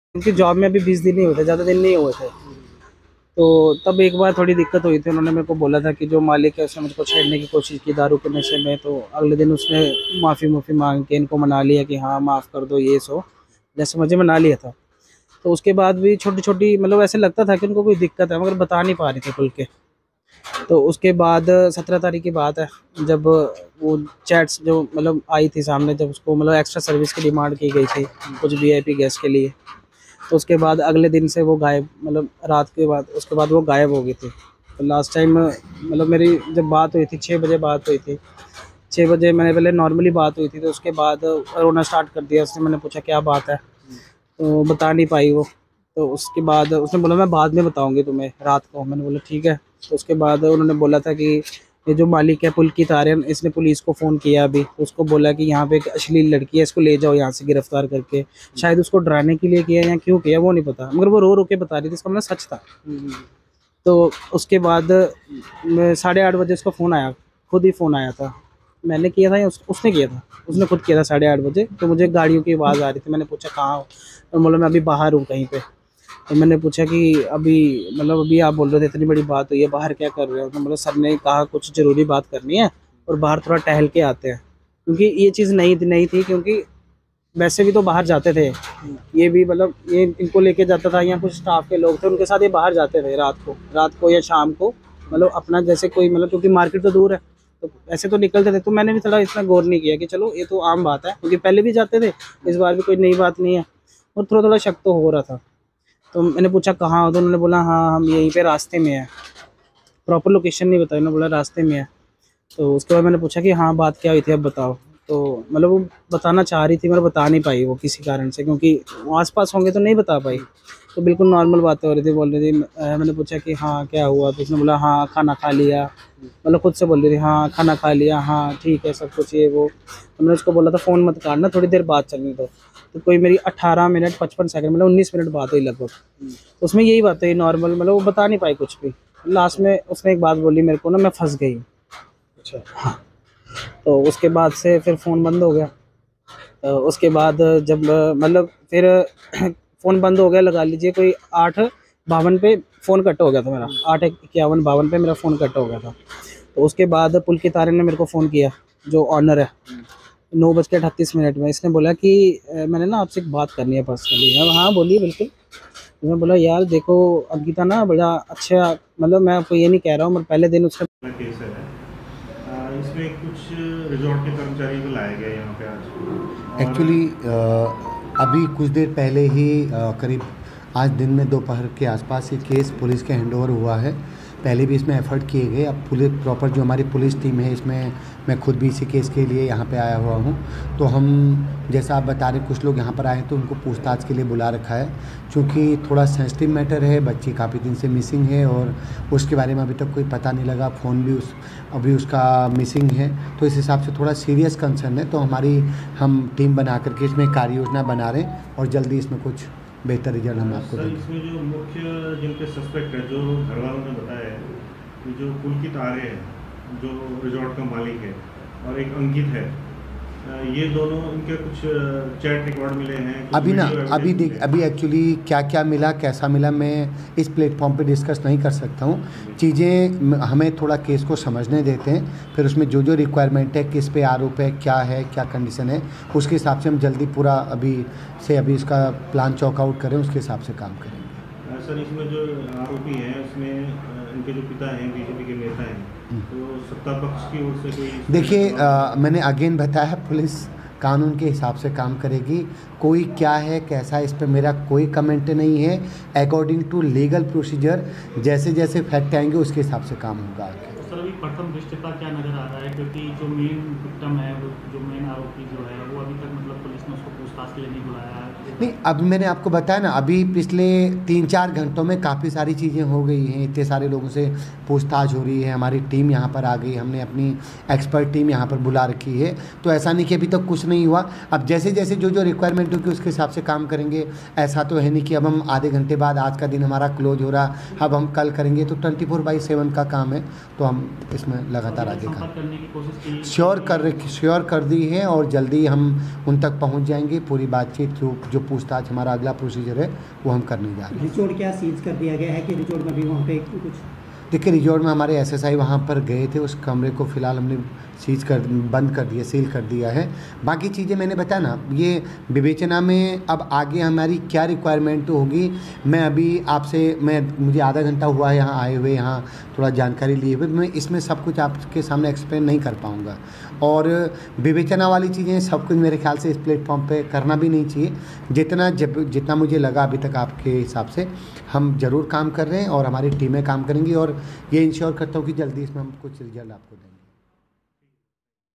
JusticeforAnkita अंकिता भंडारी के दोस्त ने बताई सच्चाई ओर क्या कहा पौड़ी के ASP शेखर सुयाल ने ।
निशब्द हूं। बस इतना कहना चाहता हूं अगर आज अंकिता को न्याय नहीं मिलेगा तो कल को ये घटना उत्तराखंड मे आम घटना बनने मे देर नहीं लगेगी। ओर कल को यही सब हमारे साथ या हमारी मां बहनों के साथ भी हो सकती है इसलिए आवाज उठाओ | मुजरिम को सजा दिलाओ अंकिता को न्याय दिलाओ।

ankita bhandari,ankita bhandari case,ankita